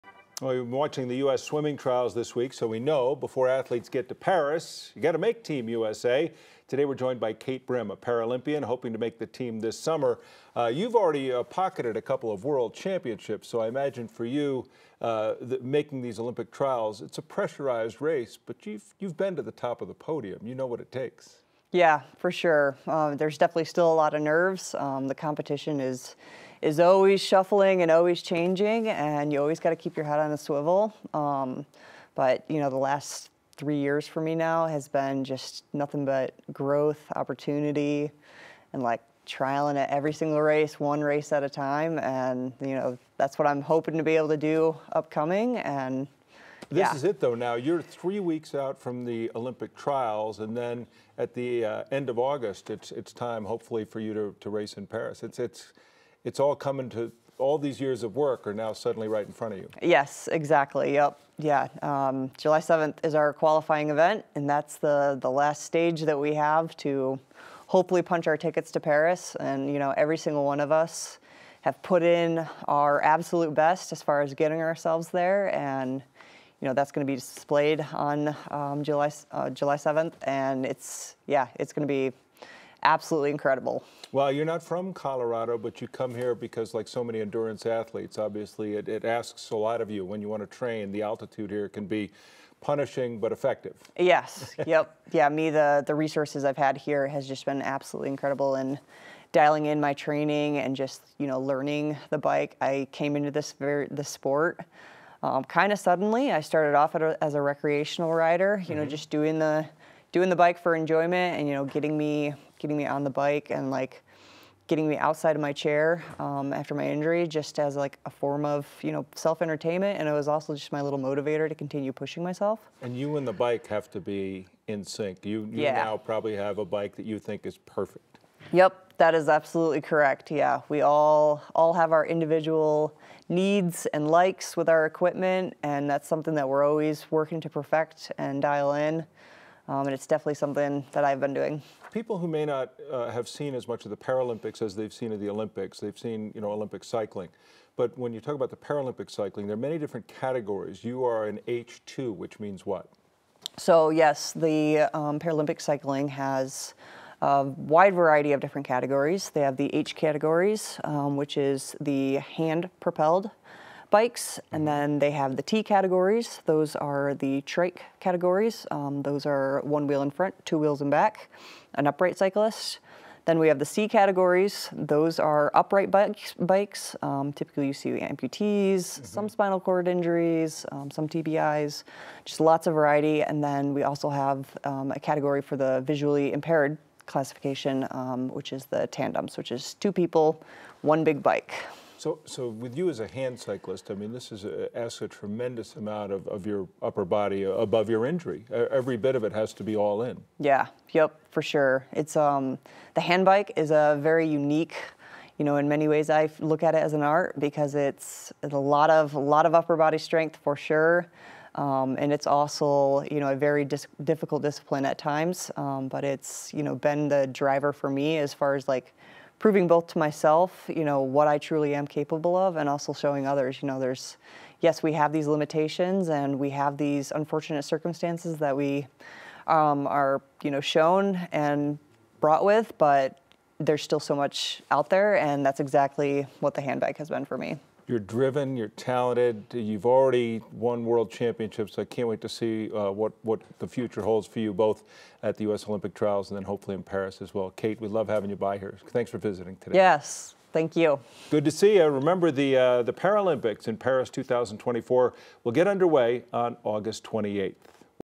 Handcyclist Kate Brim prepares for Paralympic trials
Kate Brim is no stranger to the winners' podium.

9NEWS (KUSA) is located in Denver, Colorado.